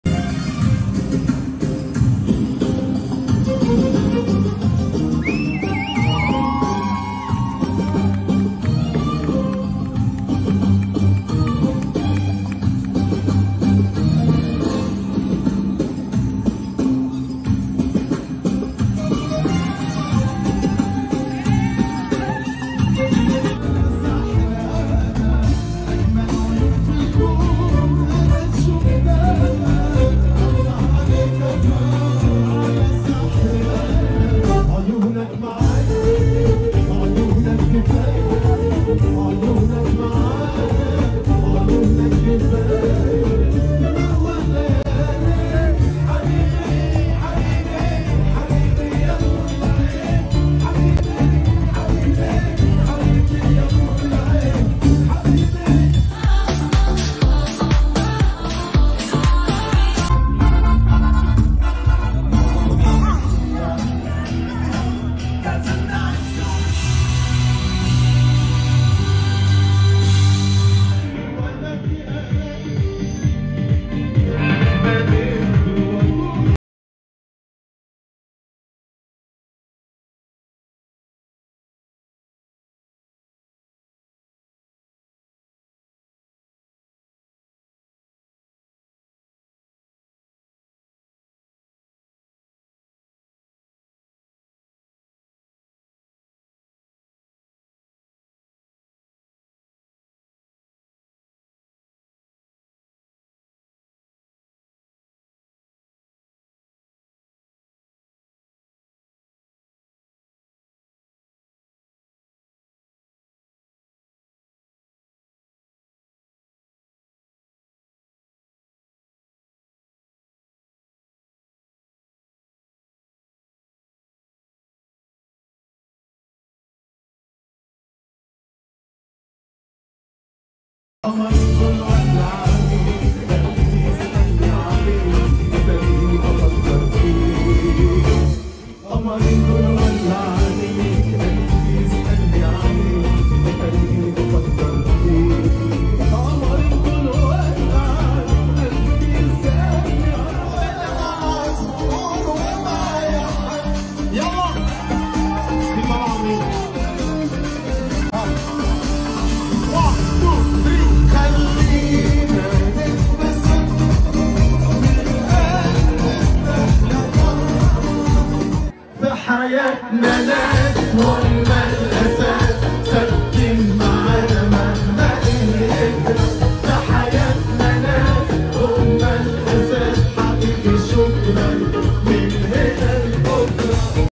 حفل زفاف مايا رعيدي ملكة جمال لبنان السابقة في أثينا اليونان بحضور عمرو دياب😍
النجم الكبير عمرو_دياب يحيي حفل زفاف مايا_رعيدي في أثينا الآن
ملكة جمال لبنان السابقة مايا_رعيدي في حفل زفافها في أثينا في اليونان الليلة
 زفاف ملكة جمال لبنان
حفل زفاف ملكة جمال لبنان
فرح ملكة جمال لبنان.